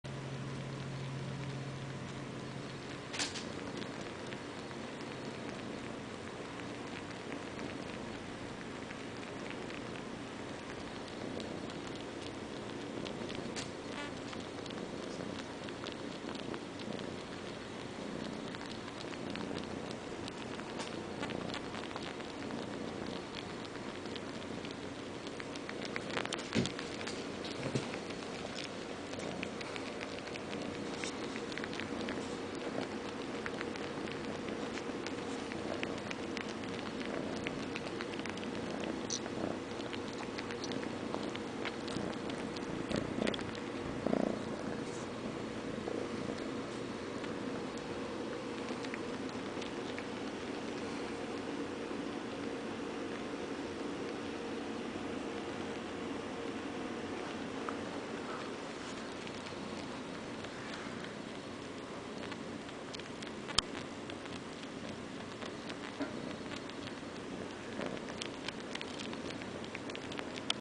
Blanket Nipple
Kylie Indie Crunch sucking on her blanket.